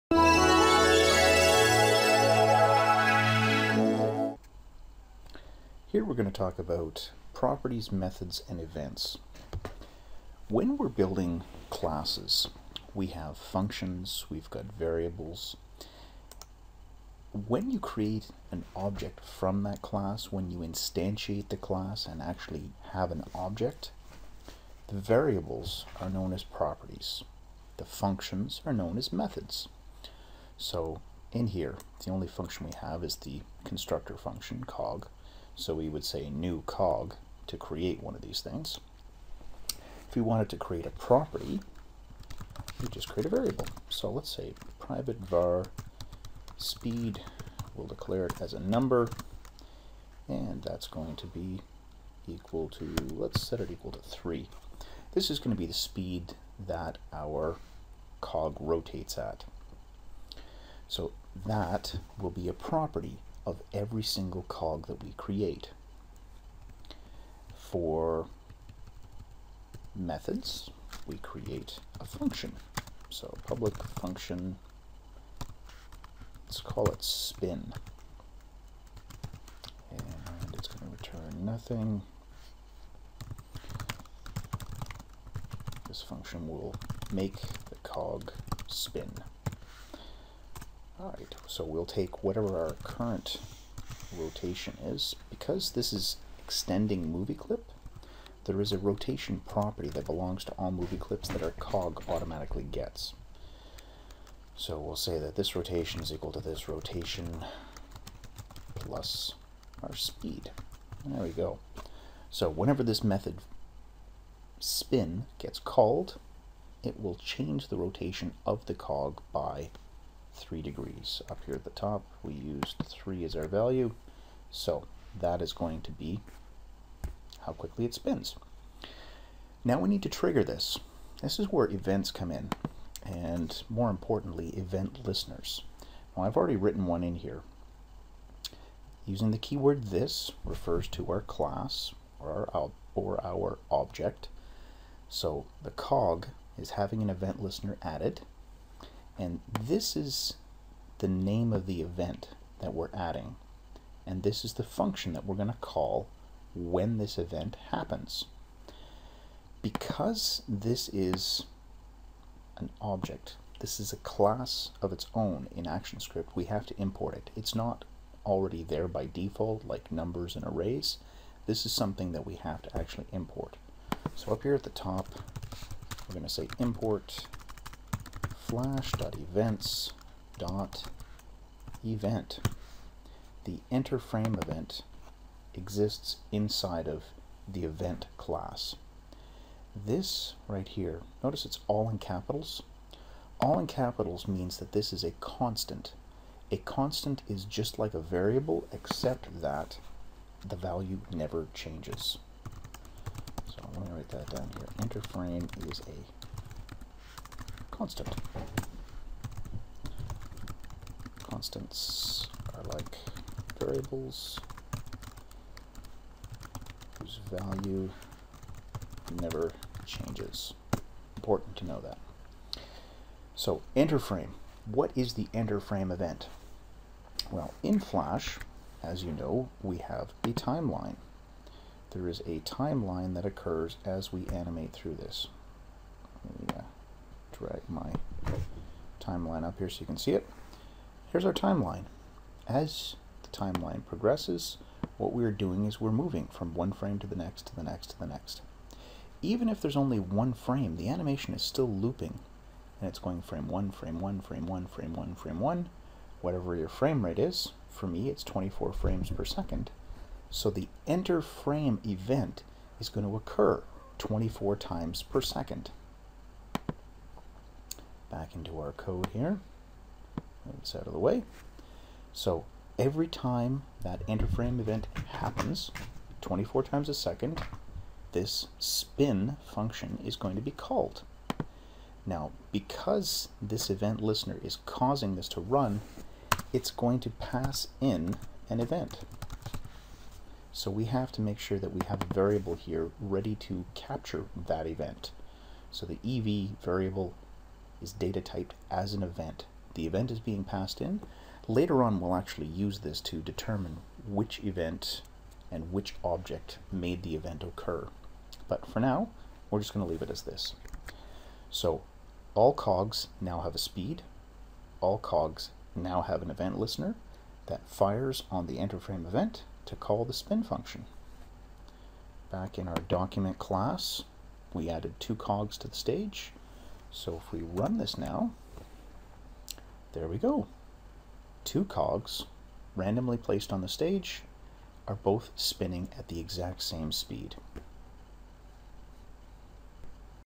MAD9021 - ActionScript Properties Methods and Events
How you create properties, methods and events in an ActionScript 3 Class. Properties are variables, methods are functions, and event listeners create the coupling between objects and events.